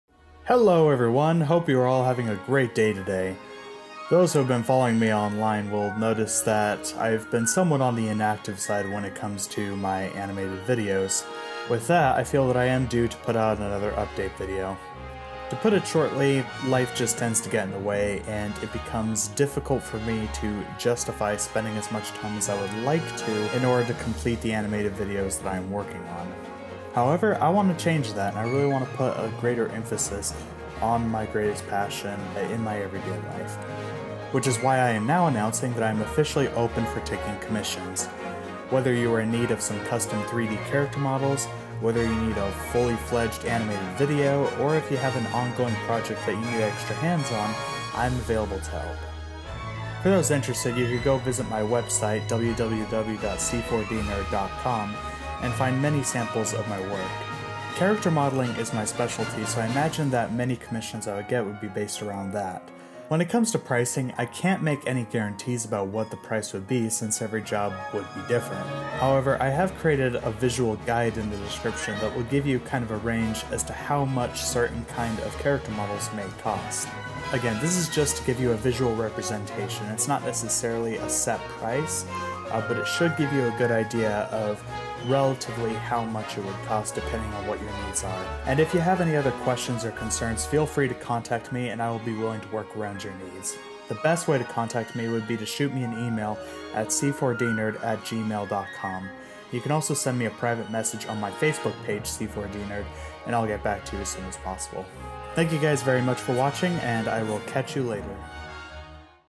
General Update - Commissions are now Open!
Just so this is made more aware, I am now open to accepting commissions and various jobs.